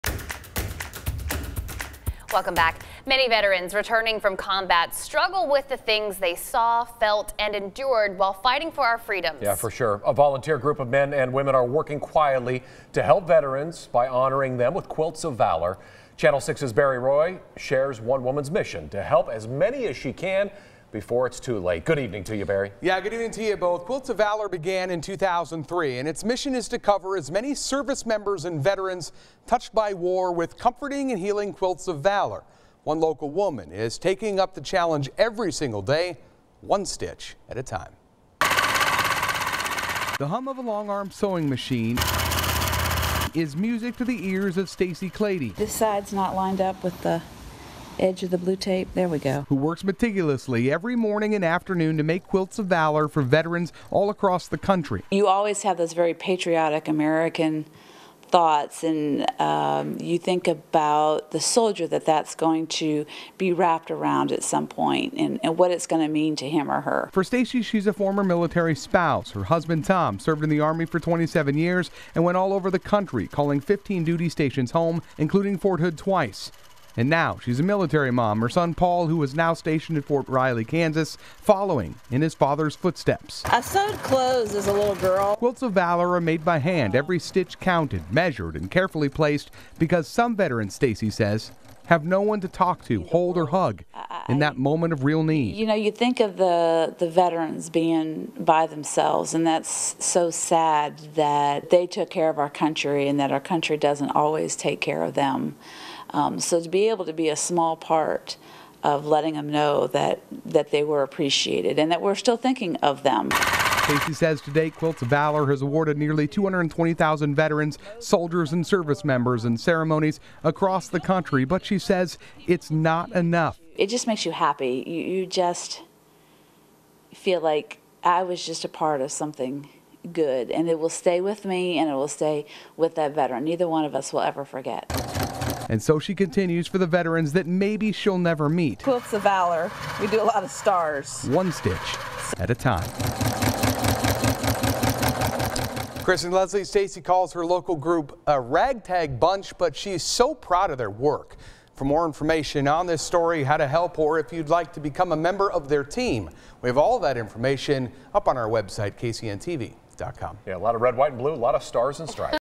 Heart and Soul: Quilts of Valor
A volunteer group works quietly to help veterans by honoring them with Quilts of Valor. See how one woman pours her heart and soul into each stitch.